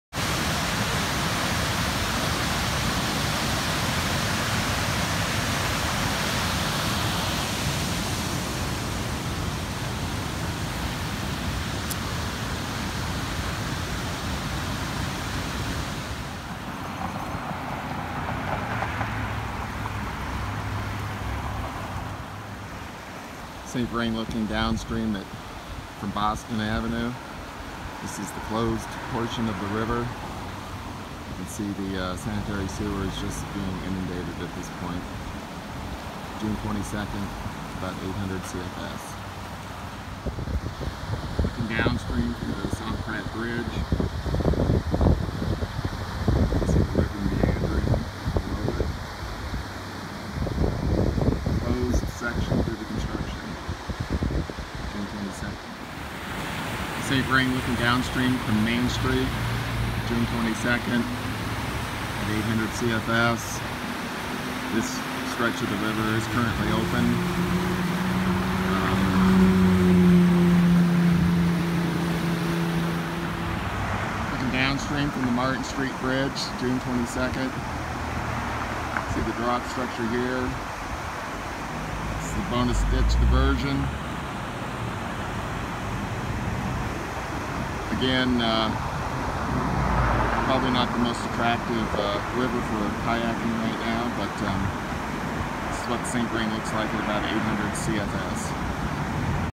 St Vrain River through Longmont June 22 2019
Flows are roughly 800 cfs. Views of river starting at Golden Ponds down to Martin Street bridge.